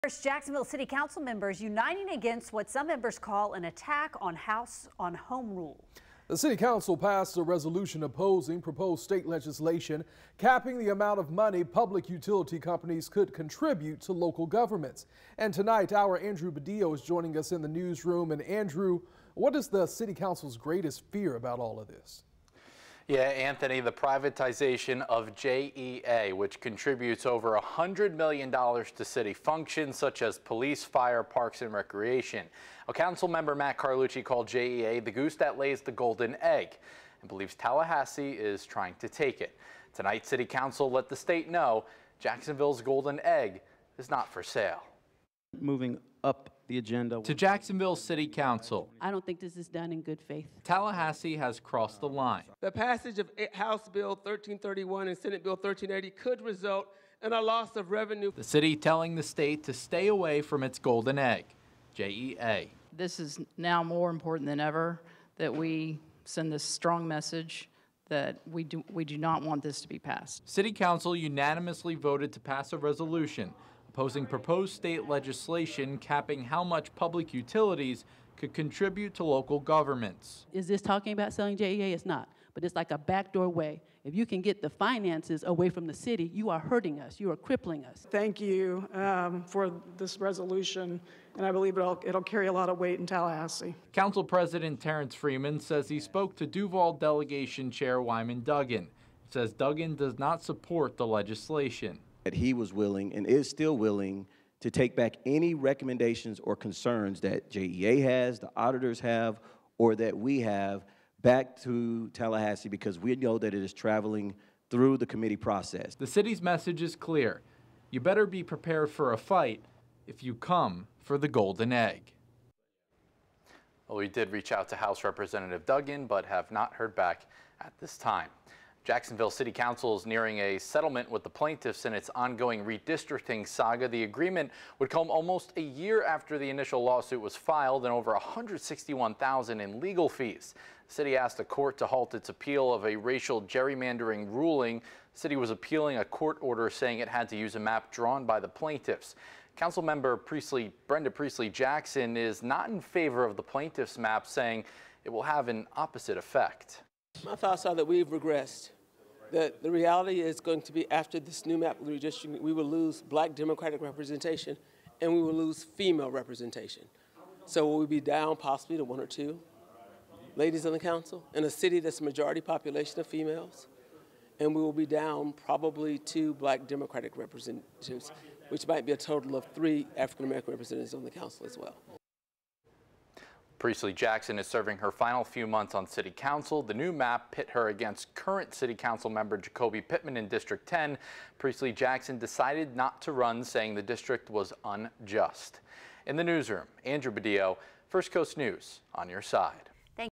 Jacksonville City Council opposed to House Bill that would cap how much public utilities could contr
HB 1331 and SB 1380 would cap the amount of money public utilities could contribute to city government functions.